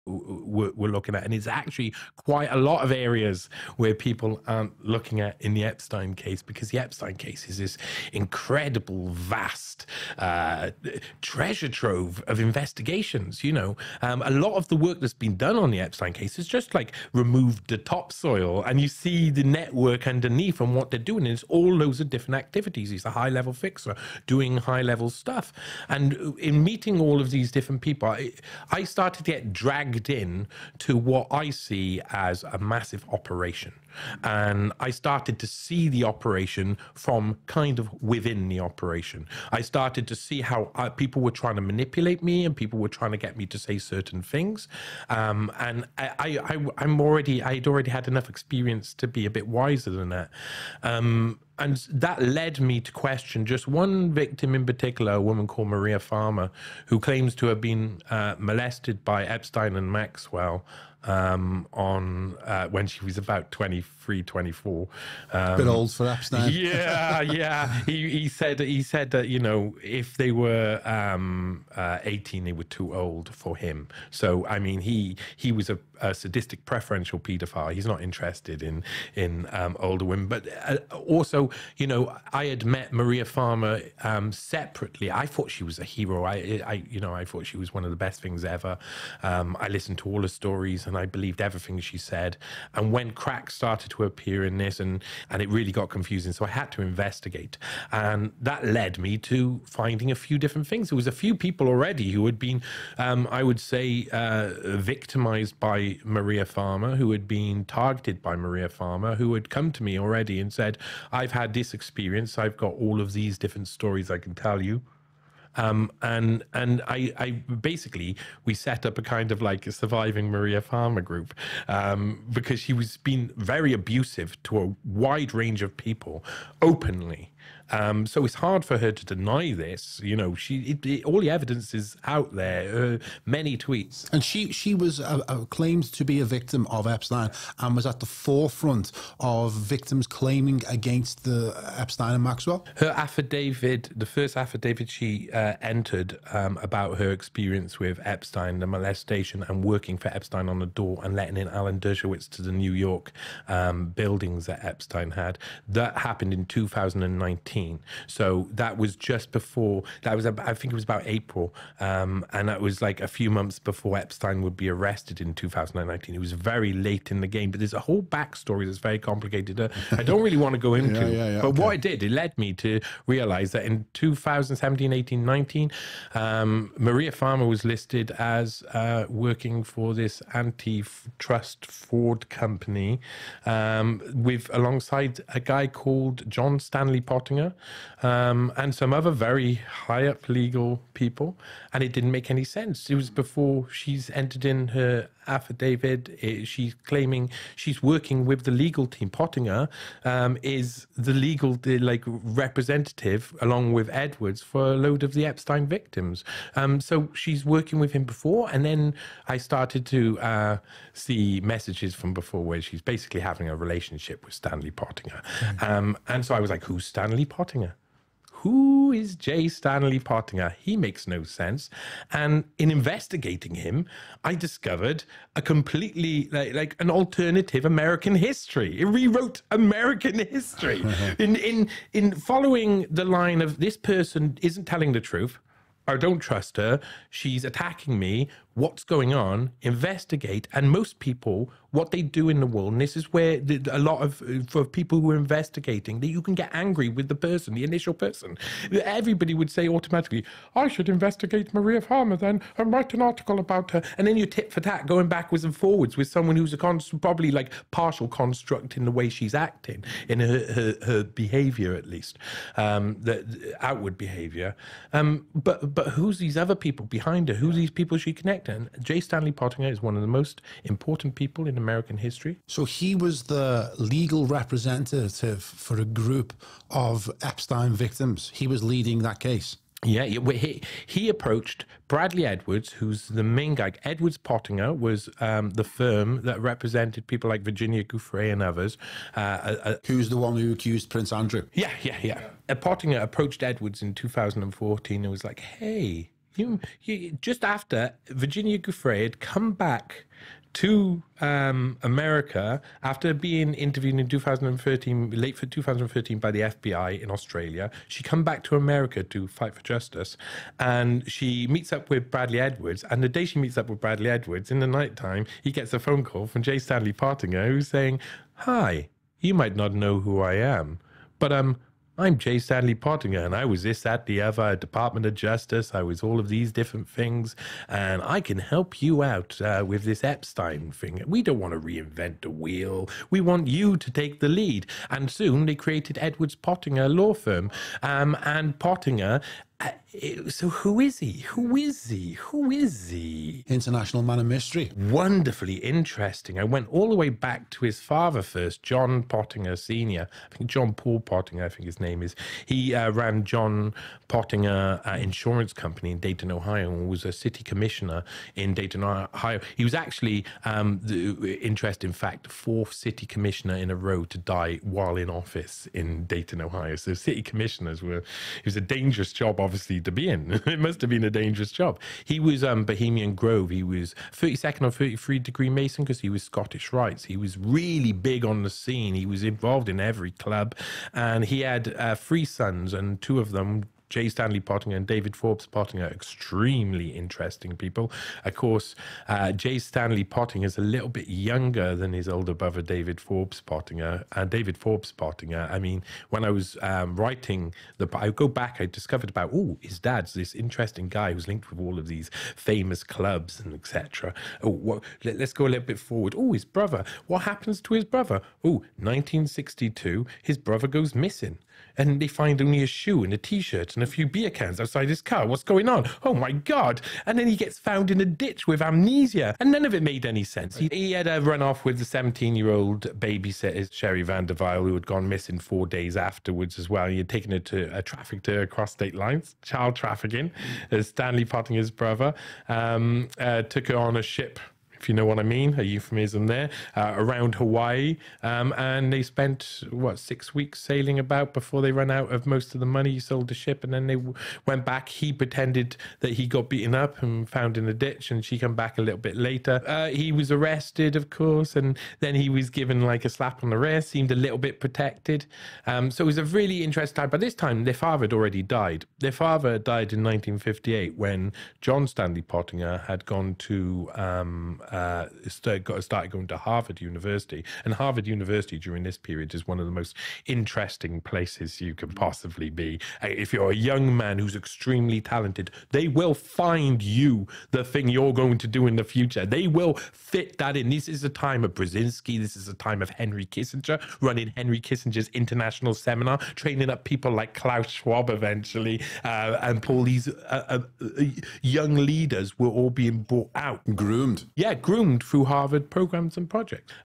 Investigating Stanley Pottinger's Close Relationship with Jeffrey Epstein - @JohnnyVedmore Clips
@JohnnyVedmore explains to @algarveamplified why he began investigating J. Stanley Pottinger who was then one of the lead lawyers for the bulk of Jeffrey Epstein's victims. He soon uncovered Epstein and Pottinger had several close ties.